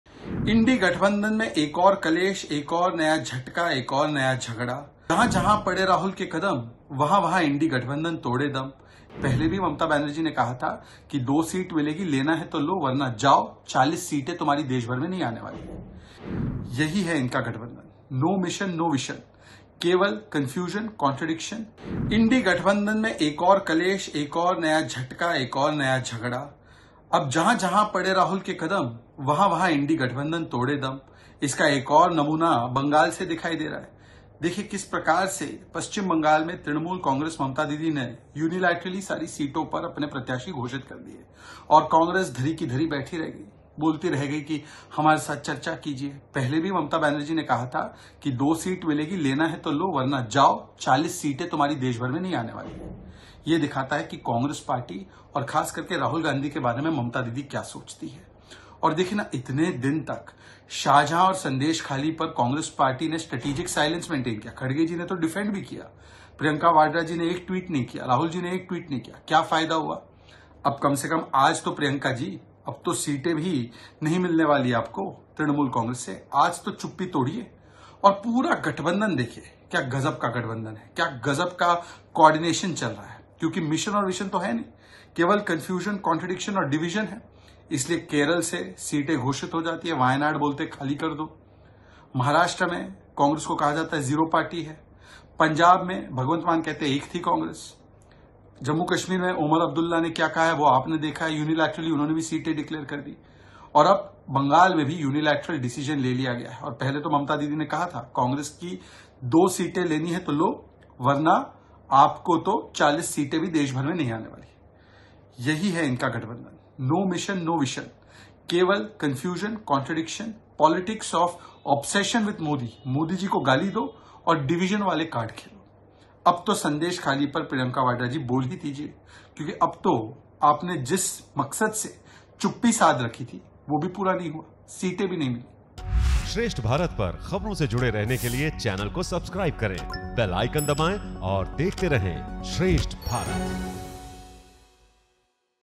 Shehzad Poonawalla ने Rahul Gandhi पर बोला हमला | BJP vs Cong | Loksabha Election 2024
BJP नेता शहजाद पूनावाल ने कांग्रेस  'INDI' गठबंधन पर हमला बोला। उन्होंने कहा,  'राहुल गांधी ने जहां रखा कदम वहां कांग्रेस और 'INDI' ने तोड़ा दम' 'ये राहुल की भारत जोड़ो न्याय यात्रा नहीं, बल्कि कांग्रेस की अंतिम यात्रा है' 'राहुल जैसे-जैसे आगे बढ़ते गए गठबंधन के साथी साथ छोड़ते गए'